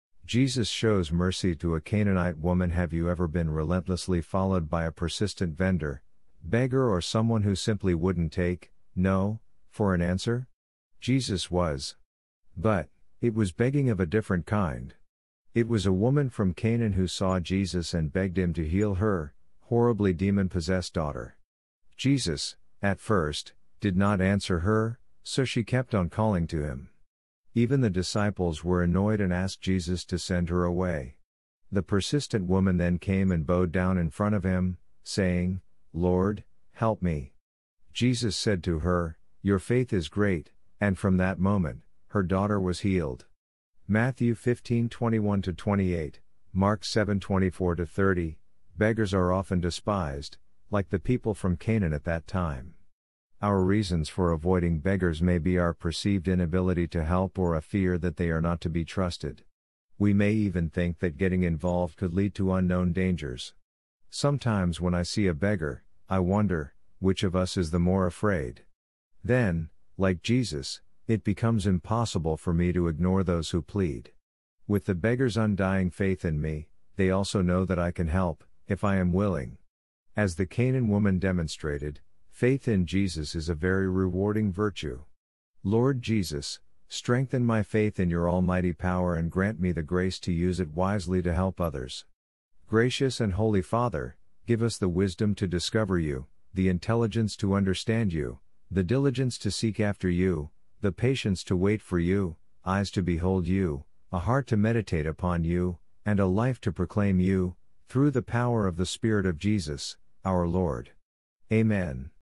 Jesus Shows Mercy To A Canaanite Woman
Have you ever been relentlessly followed by a persistent vendor, beggar or someone who simply wouldn’t take, “NO”, for an answer?
 Jesus was! But, it was begging of a different kind. It was a woman from Canaan who saw Jesus and begged Him to heal her, “Horribly demon-possessed daughter”. Jesus, at first, did not answer her, so she kept on calling to him. Even the disciples were annoyed and asked Jesus to send her away.
 The persistent woman then came and bowed down in front of Him, saying, “Lord, help me!”
 Jesus said to her, “Your faith is great”, and from that moment, her daughter was healed. (Matthew 15:21-28, Mark 7:24-30)
 Beggars are often despised, like the people from Canaan at that time.
 Our reasons for avoiding beggars may be our perceived inability to help or a fear that they are not to be trusted. We may even think that getting involved could lead to unknown dangers.
 Sometimes when I see a beggar, I wonder, which of us is the more afraid! Then, like Jesus, it becomes impossible for me to ignore those who plead. With the beggar’s undying faith in me, they also know that I can help, if I am willing.
 As the Canaan woman demonstrated, faith in Jesus is a very rewarding virtue.
 Lord Jesus, strengthen my faith in your almighty power and grant me the grace to use it wisely to help others.
 Gracious and Holy Father,give us the wisdom to discover You,the intelligence to understand You,the diligence to seek after You,the patience to wait for You,eyes to behold You,a heart to meditate upon You,and a life to proclaim You,through the power of the Spirit of Jesus, our Lord. Amen.